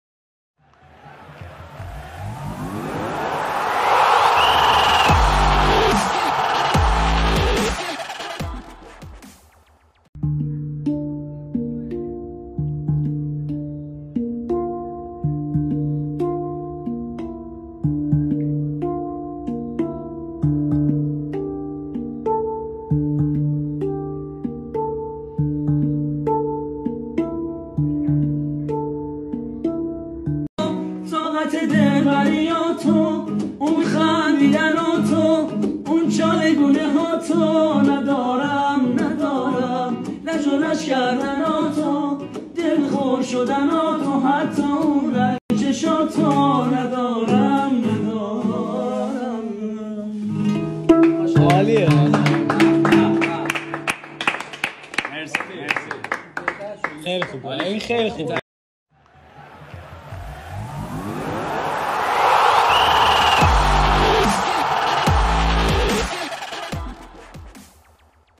خوانندگی و نوازندگی دلنشین پورعلی گنجی 😳😍
کی فکرشو میکرد مرتضی پور گنجی، مدافع مستحکم تیم ملی فوتبال ایران  و السد قطر همچین دل نازک و عاطفه ای داشته باشه! 😍 با هم ببینیم و بشنویم هنگ درام نوازی و در ادامه خوانندگی مرتضی پور علی گنجی رو!

مارو حتما در اینستاگرام دنبال کنید 📺🔥
Instagram.com/Sorkhabii.TV
کانال تلگرامی سرخابی 🌏
Telegram.me/Sorkhabiitv